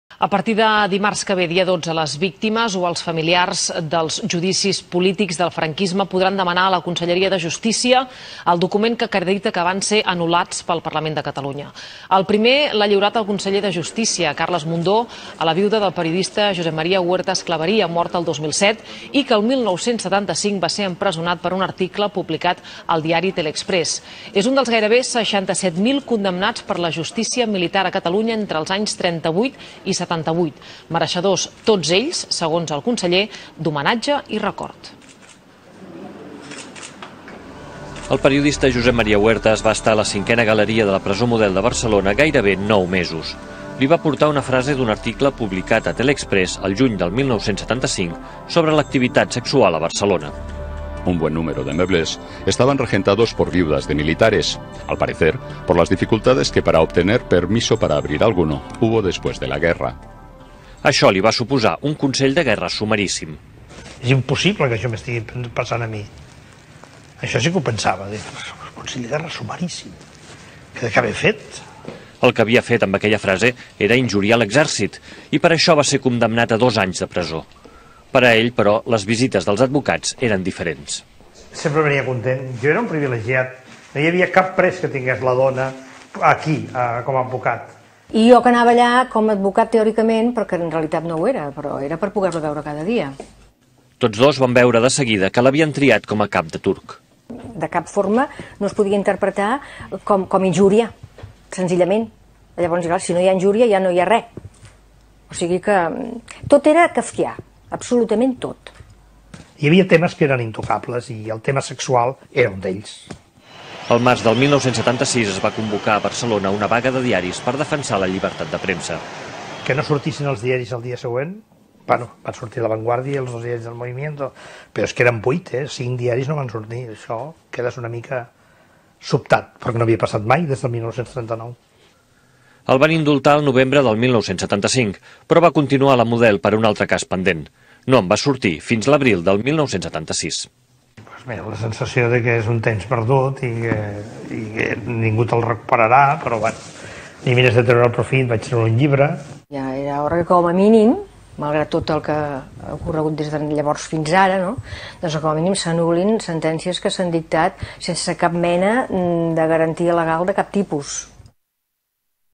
Carles Mundó entrega avui el primer document que acredita la nul·litat d'un judici franquista
El conseller de Justícia, Carles Mundó, entrega avui el primer document que acredita la nul·litat d'un judici polític durant el franquisme a Araceli Aiguaviva, vídua del conegut periodista i escriptor Josep Maria Huertas Claveria.
Tv3. Anul·lació Judici Franquistes. 8 de setembre de 2017